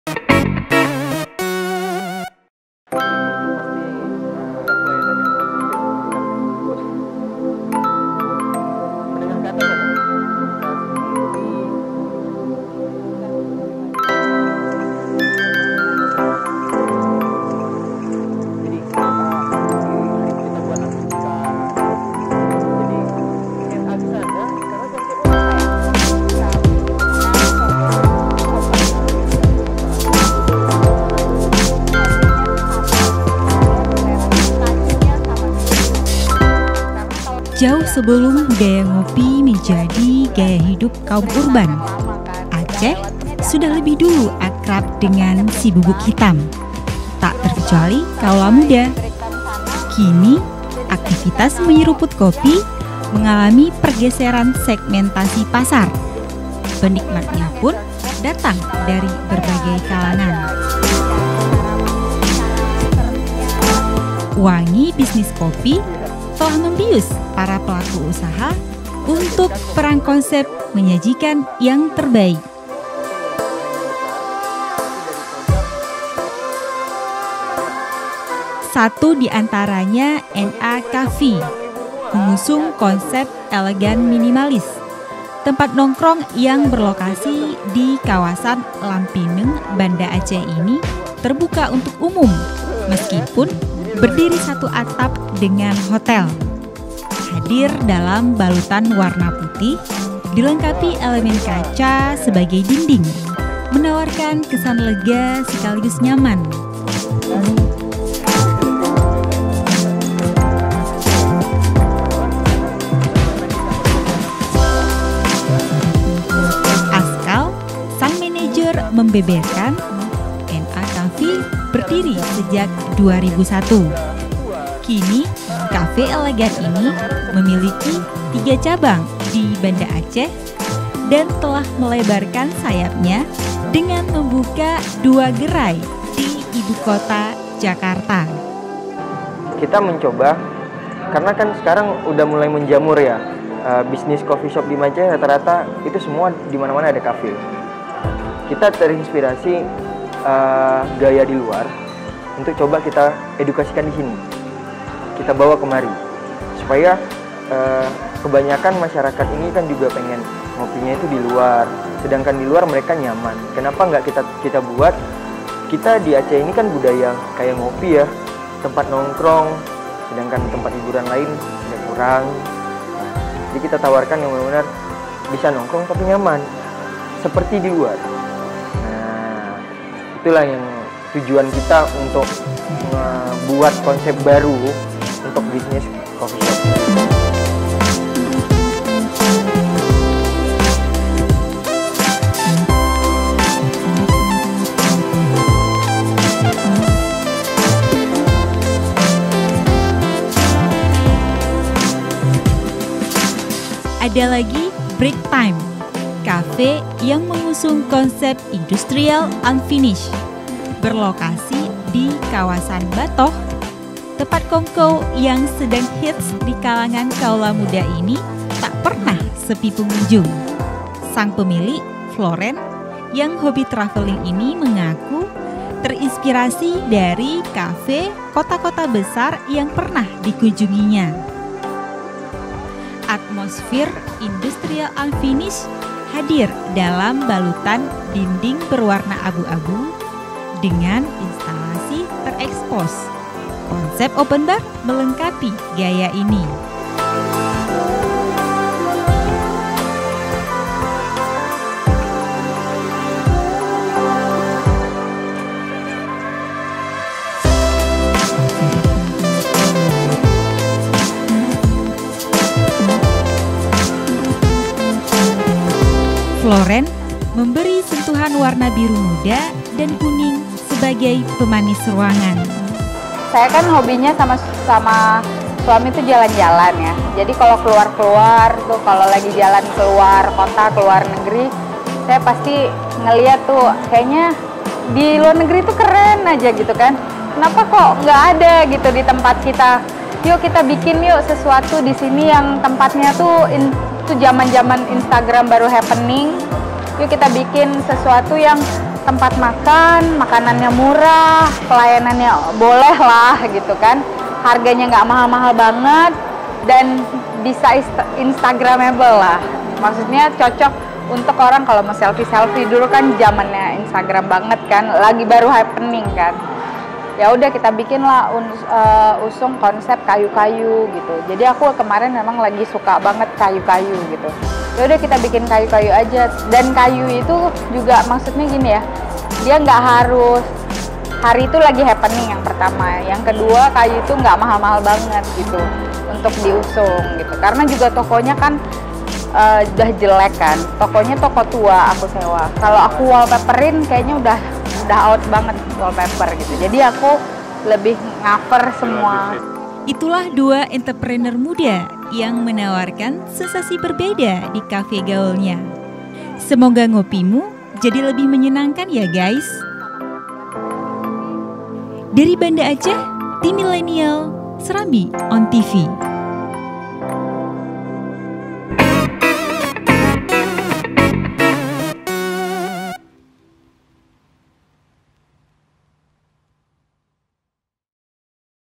'Perang' Konsep Ala Enterpreneur Muda
SERAMBITV.COM, BANDA ACEH -Jauh sebelum budaya ngopi menjadi gaya hidup kaum urban, Aceh sudah lebih dulu akrab dengan 'si bubuk hitam'.

Tak terkecuali kawula muda. Kini aktivitas menyeruput kopi, mengalami pergeseran segmentasi pasar.

Penikmatnya pun datang dari berbagai kalangan.

Wangi bisnis kopi, telah membius para pelaku usaha untuk 'perang konsep' menyajikan yang terbaik.

Satu di antaranya, NA Coffee. Mengusung konsep elegan  minimalis, tempat nongkrong yang berlokasi di Kawasan Lampineung, Banda Aceh ini terbuka untuk umum meskipun berdiri satu atap dengan hotel.

Hadir dalam balutan warna putih dilengkapi elemen kaca sebagai dinding, menawarkan kesan lega sekaligus nyaman.

Askal (26), sang manajer membeberkan, NA Coffee berdiri sejak 2001.

Kini kafe elegan ini memiliki tiga cabang di Banda Aceh dan telah melebarkan sayapnya dengan membuka dua gerai di ibukota, Jakarta.

Adalagi Break Time, kafe yang mengusung konsep industrial unfinished.

Berlokasi di Kawasan Batoh, tempat kongkow yang sedang hits di kalangan muda ini tak pernah sepi pengunjung.

Sang pemilik, Floren (27) yang hobi traveling ini mengaku terinspirasi dari kafe kota-kota besar yang pernah dikunjunginya.

Atmosfer industrial unfinished hadir dalam balutan dinding berwarna abu-abu, dengan instalasi tereskpos.

Konsep open bar melengkapi gaya ini. Floren memberi sentuhan warna biru muda dan kuning sebagai pemanis ruangan.

Itulah dua enterpreneur muda yang menawarkan sensasi berbeda di kafe gaulnya. Semoga ngopimu jadi lebih menyenangkan ya guys!